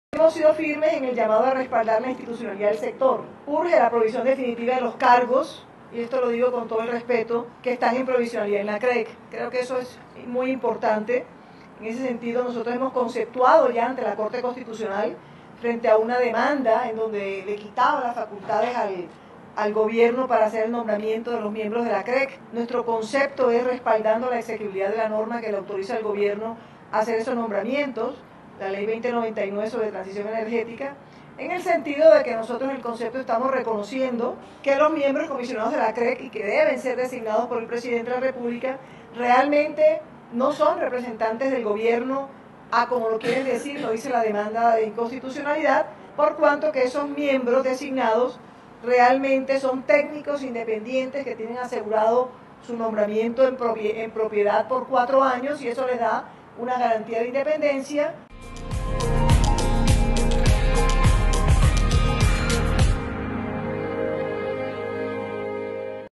Miembros comisionados de la CREG deben ser técnicos, independientes y autónomos: Procuradora
¡Garantizando la independencia en la regulación energética! 💡💪 La Procuradora MargaritaCabello destaca la estabilidad de los agentes expertos que hacen parte de la Comisión de Regulación de Energía y Gas por un período de 4 años.